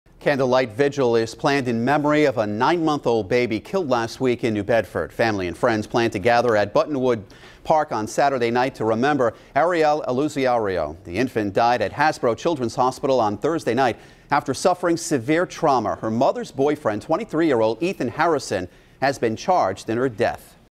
Candlelight vigil to remember infant
A candlelight vigil is planned in memory of a nine-month old baby killed last week in New Bedford.